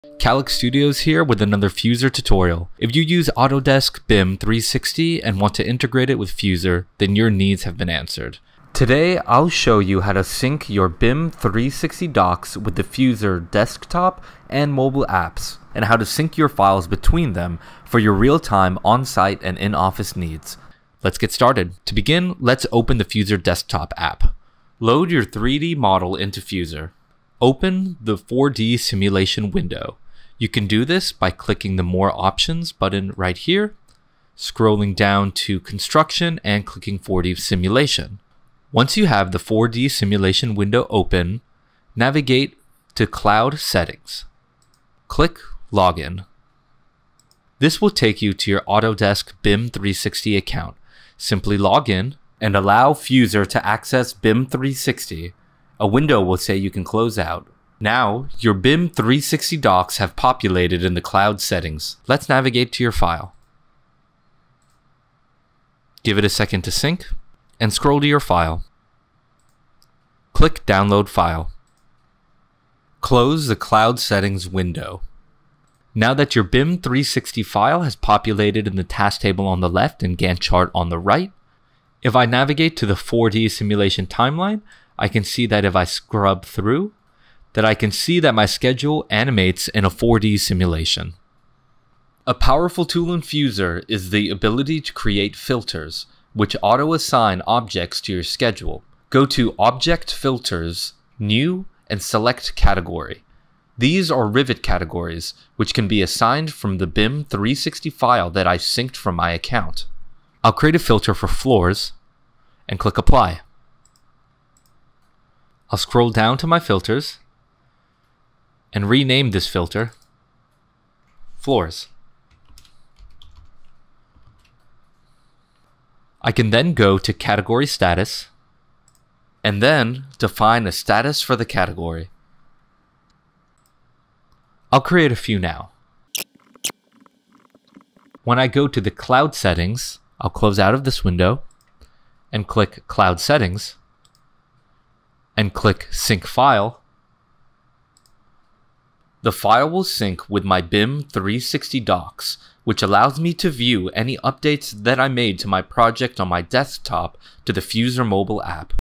How to Connect BIM360 & Fuzor Workstation
Upload Fuzor 4D model to BIM 360 for project status update